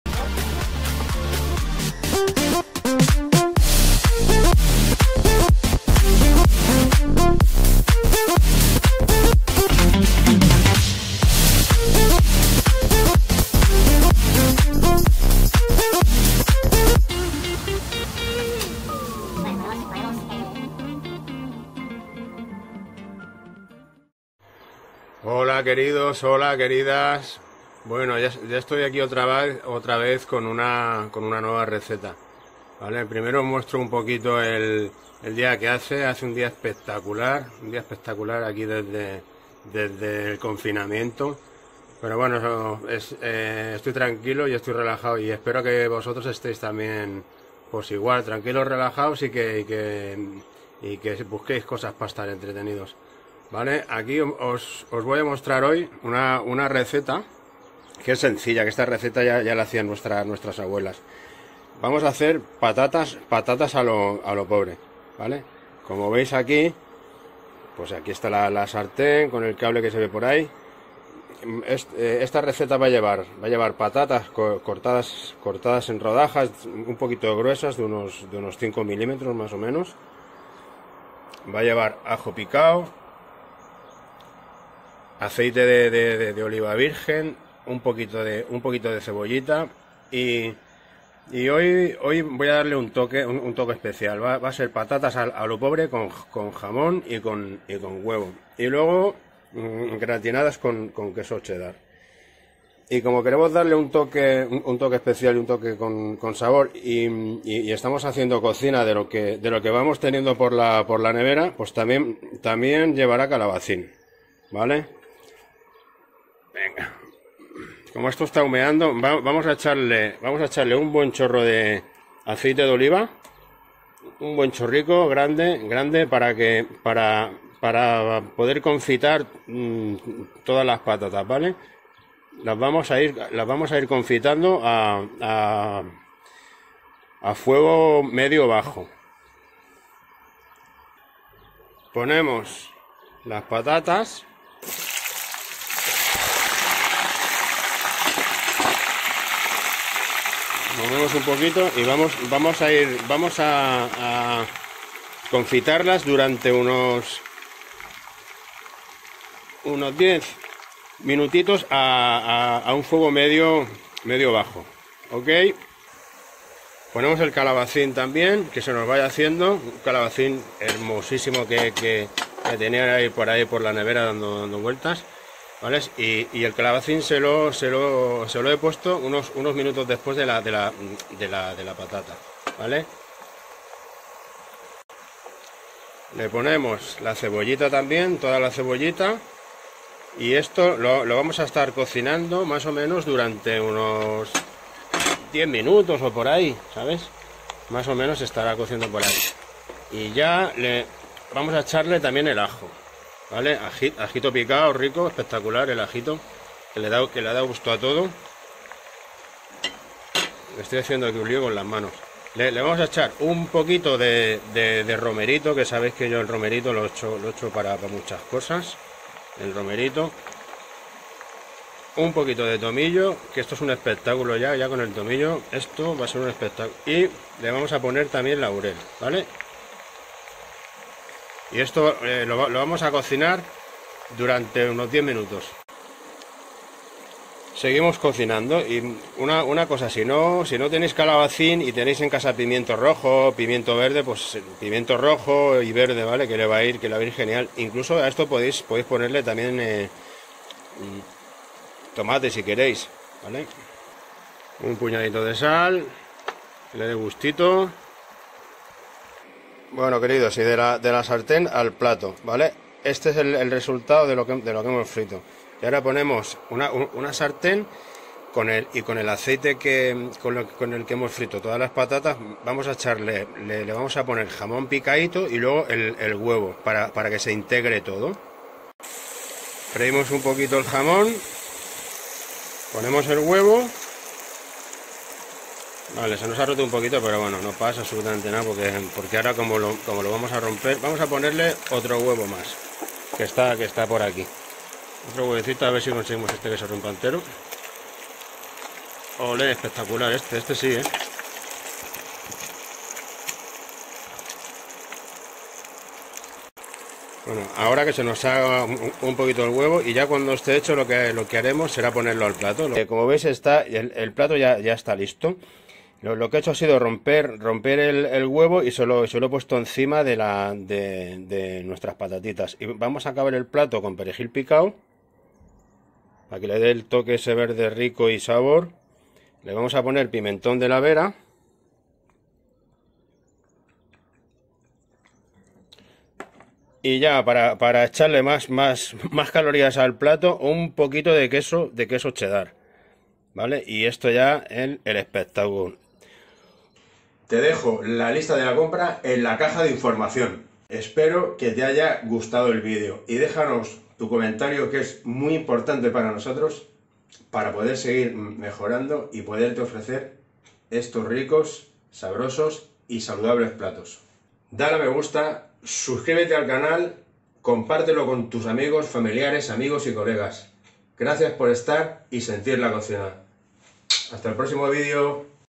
PATATAS A LO POBRE.+Tradicional¡ Te haran llloran de emoción☘☘
PATATAS a lo Pobre¡¡¡🟢*Guarnicion ¡¡con HUEVO y Jamon. Plato rápido cocinando salud. COCINANTES¡¡¡ En este video os muestro *COMO HACER PATATAS A LO POBRE *con Jamon y Huevo Revuelto* una receta de NUESTRA COCINA FACIL¡¡¡de elaborar SALUDABLE y con una resultado que te sorprendera por su sabor y su sencillez. TE SIRVE¡¡ como primer plato COMO guarnicion o PLAT0 UNICO..por los elementos que aporta.APTA para alimentacion Vegana y Vegetariana sustituyendo o eliminando los ingredientes que no desees.
SORPRENDE * a los TUYOS con esta SABROSA receta...
GRACIAS por Estar y SENTIR la COCINA:**

OS DEJO POR AQUI LA LISTA DE LA COMPRA¡¡¡¡¡¡¡

Para dos personas::
_ 5 Patatas o Papas medianas
_ Medio calabacin

_ Una pizca de tomillo y romero

_ 2 huevos
_ 2 lonchas de jamon

_ 50 grs de Queso Chedar
_ Perejil picado

DEJADNOS VUESTROS COMENTARIOS ¡¡ NOS AYUDAREIS A MEJORAR.